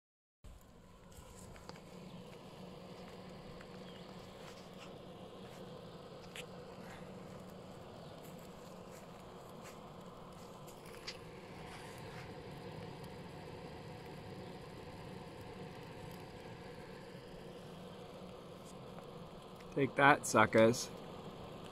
Skeetervac in action
Skeetervac killing black flies in Nova Scotia.
Found this Skeetervac in the garbage, replaced the propane hose.
Fired right up so I bought the sticky paper and then sat back and watched the blackflies die.